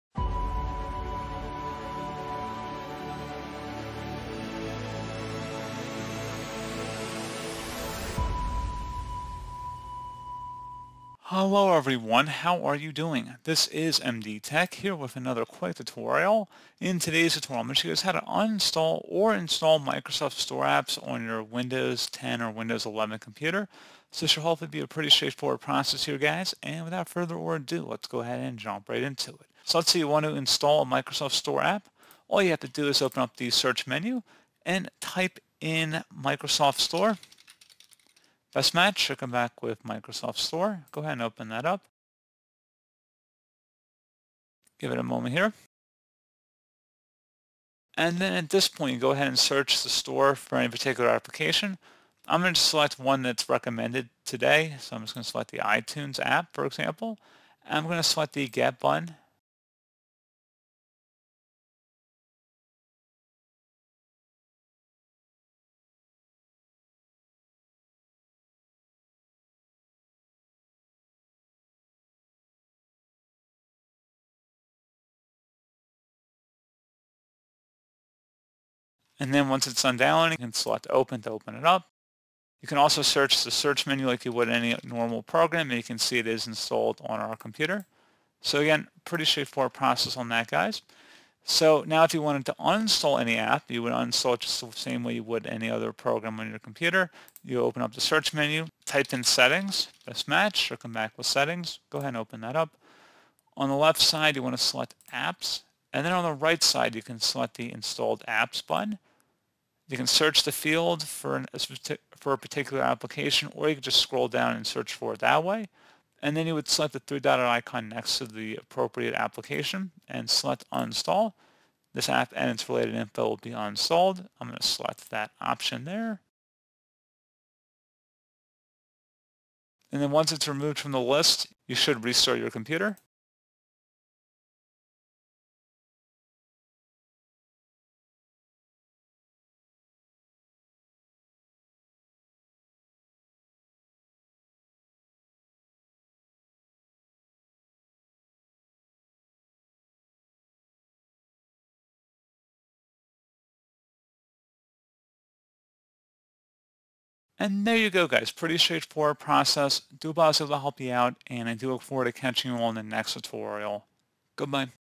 How to Install or Uninstall Microsoft Store Apps in Windows 11/10 [Guide]
This is a basic tutorial for Windows 11/10 beginners, who want to know how to install or uninstall Universal Windows Platform (UWP) apps in Windows 11/10 that you download from the Microsoft Store. The process is quite simple, so let us take a look at it.

If you wish to install Microsoft Store Apps on your Windows 11/10 PC, you will have to visit the official Microsoft Store, search for the app, and then download and install it.

Type ‘store‘ in the taskbar bar search and click open the Store app. Using the search bar, search for the app. Once the Store app is found, click on the Install button.

uninstall or remove apps and programs in windows 10

The Microsoft Store makes it easy to download and manage apps on your Windows computer. However, if the app store starts to act up, you can reinstall it to fix the problems triggered due to file corruption, temporally glitches and bad updates.